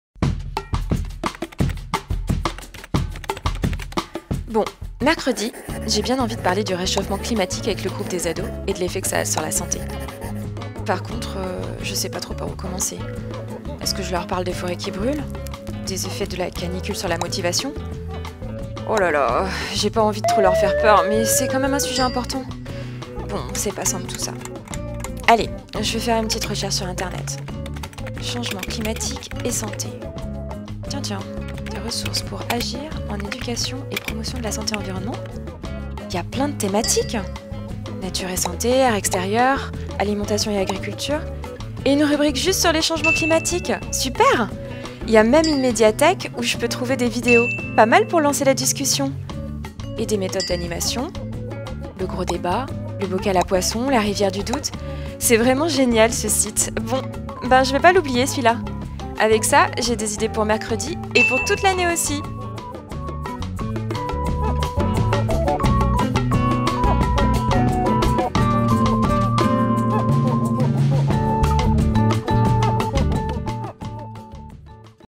Santé environnement / Jessica, animatrice, découvre le site agir-ese.org
"Jessica est animatrice au Centre social des Mimosas… elle veut aborder les changements climatiques avec son groupe de jeunes. C’est le casse-tête… comment faire ?
Le site agir-ese.org –  un site avec des ressources pour éduquer en santé-environnement – lui apporte de l’aide autant sur le fond (articles thématiques, médiathèque…) que sur la forme (méthodes d’animation…)."

Mai 2021 – GRAINE Occitanie, GRAINE ARA, IREPS ARA et IREPS Occitanie